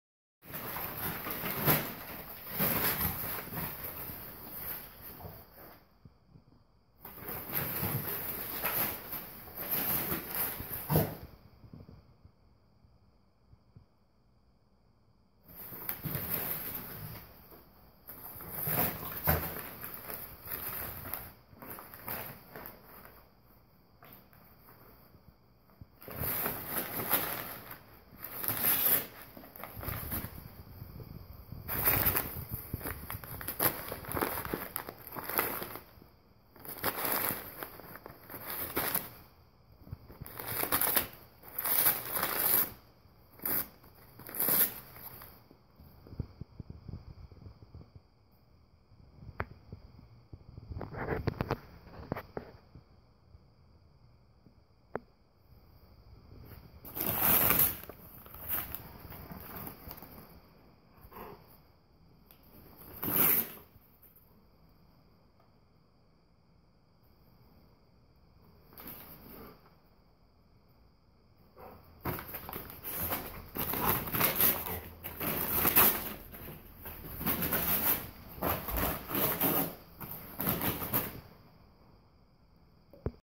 Super Funny! - Dog Rolling Around Like Crazy In Shredded Packing Paper - Golden Retriever
We let our 3 year old english cream golden retriever dog tear apart some brown packing paper and then he started rolling around in it and kicking his legs in the air.